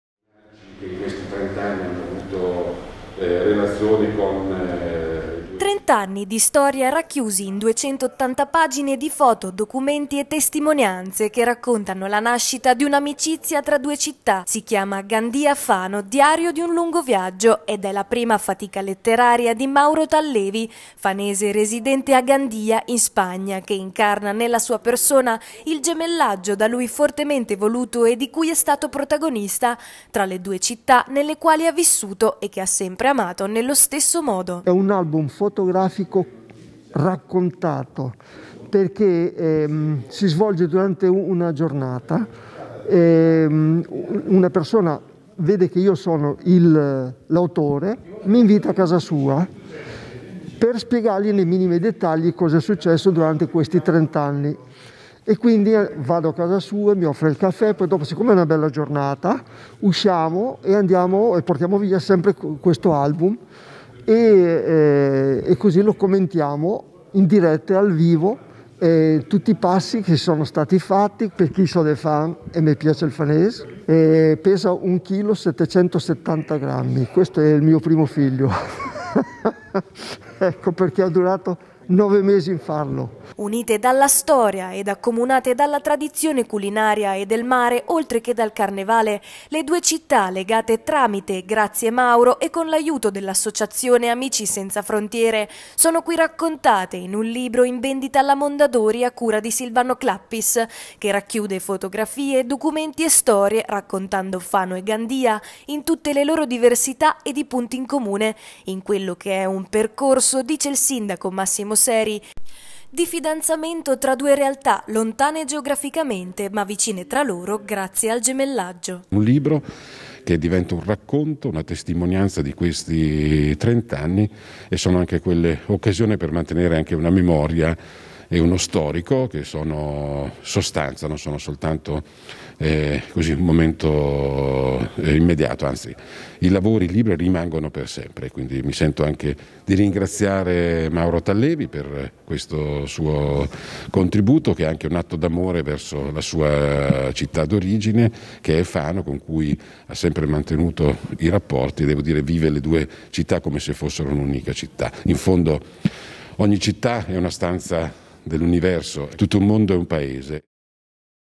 Mauro Tallevi racconta Gandia e Fano
280 pagine e 30 anni di storia raccontati tramite fotografie e aneddoti che testimoniano la nascita di un legame tra due città. Si chiama “Gandia-Fano, diario di un lungo viaggio” ed è il nuovo libro di Mauro Tallevi.